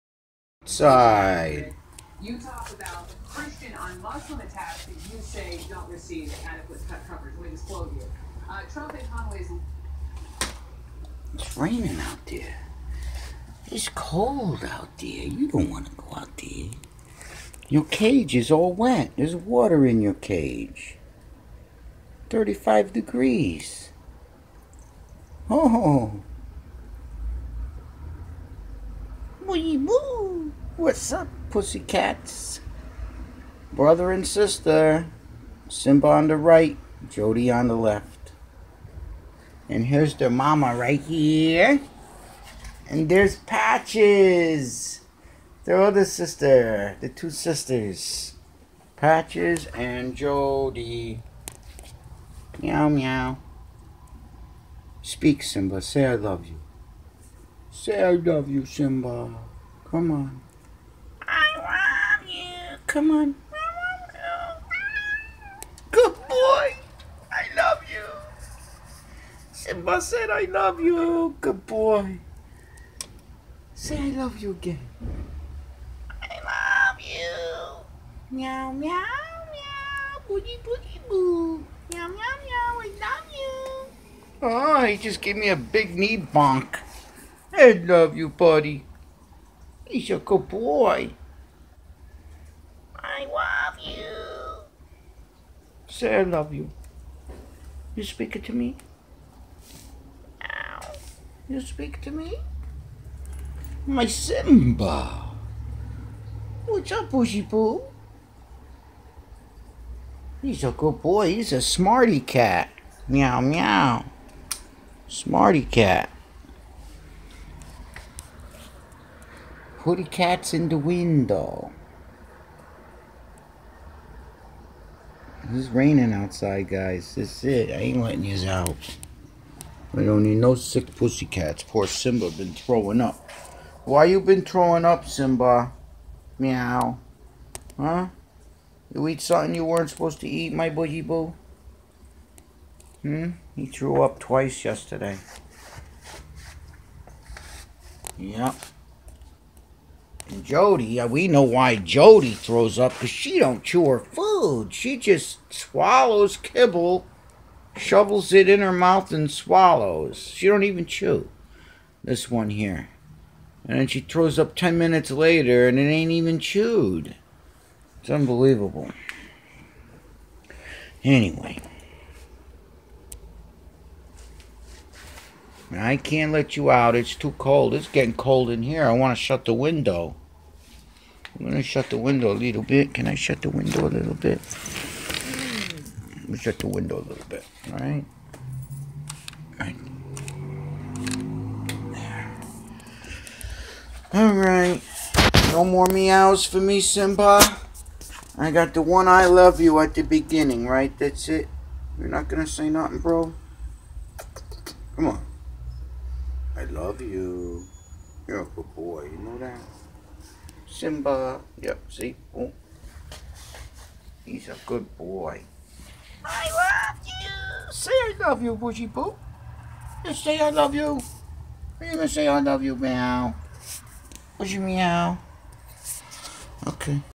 Smarty Cat Simba says I LOVE YOU
Simba does this to me just like The Great Late Tiger Kitty. Meows I LOVE YOU all the time just to get my attention  but he says it one time I LOVE YOU MEOWS !    1:04   he says it right after I say it .